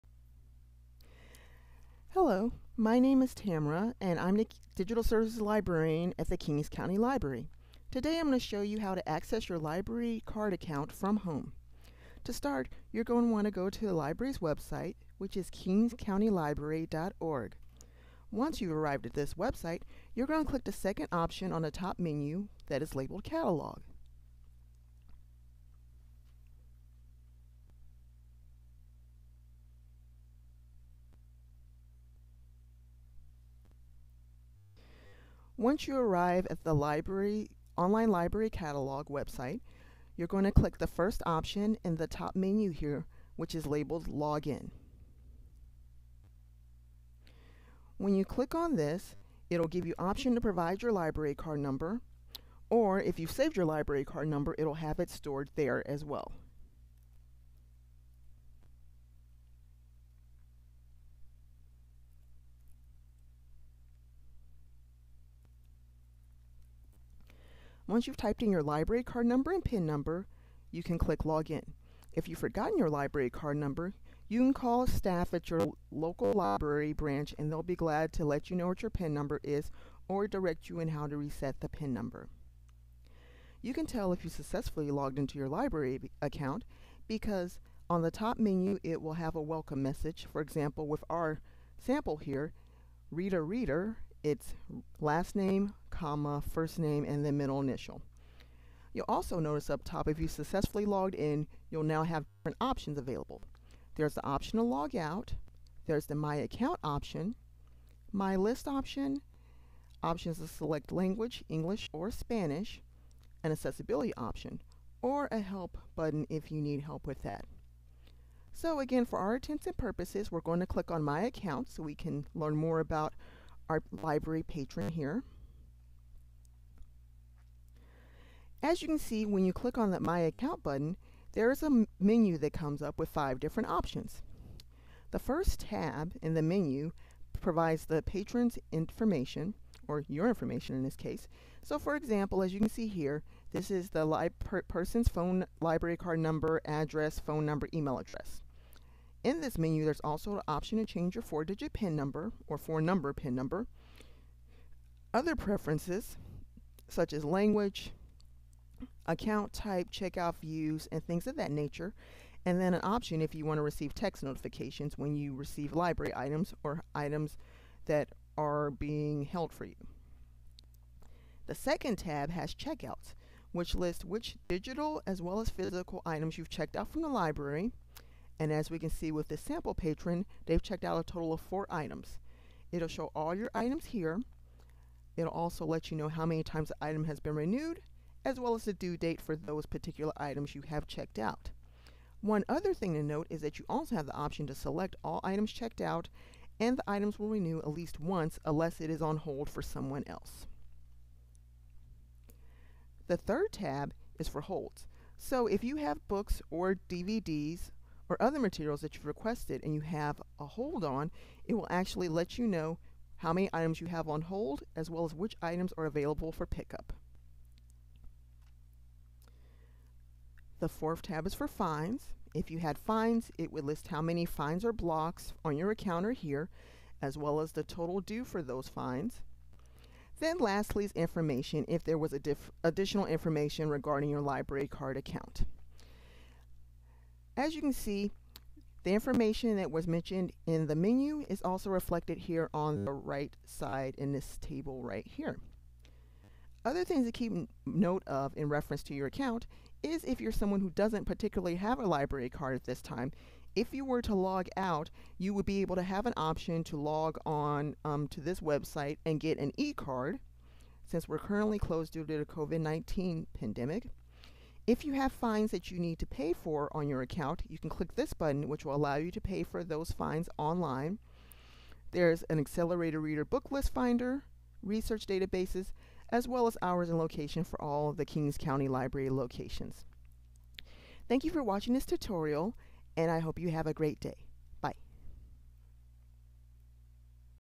Using ValleyCat To Access Library Card Account Tutorial
This is a tutorial on how to access the ValleyCat online library website as well as view your library account.